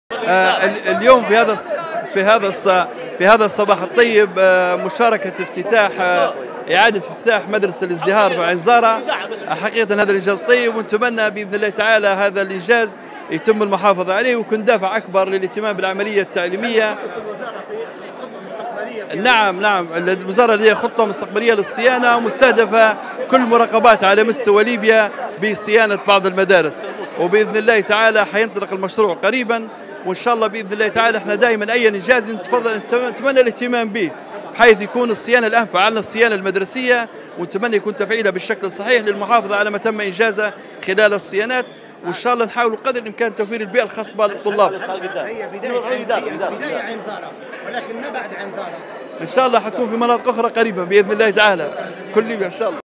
موسى المقريف: الوزارة لديها خطة مسبقة لصيانة مدارس كل المراقبات على مستوى ليبيا
خاص التناصح | موسى المقريف/ وزير التعليم بحكومة الوحدة الوطنية: الوزارة لديها خطة مسبقة لصيانة مدارس كل المراقبات على مستوى ليبيا

افتتاح مدرسة الازدهار بمراقبة تعليم عين زارة بعد الصيانة

ــــــــــــــــــــــــــــــــــــــــــــــــــــــــــــــــــــــــــ
ــــــــــــــــــــــــــــــــــــــــــــــــــــــــــــــــــــــــــ
يمكنكم تحميل تطبيقنا TANASUH TV على الهواتف الذكية: